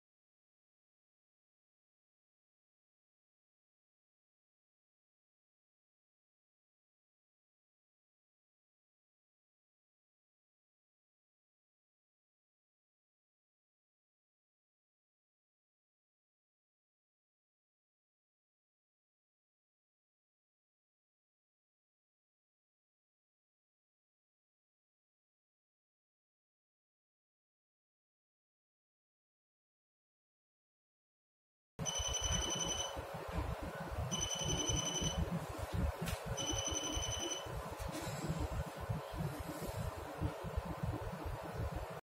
Toy traffic light
It is a toy but also a real traffic light, the kids can play it when learning the knowledge of the road trafficsafty rules.
It can work automatically switching between red/yellow/green/pedestrian ,you can also use manual mode to control by yourself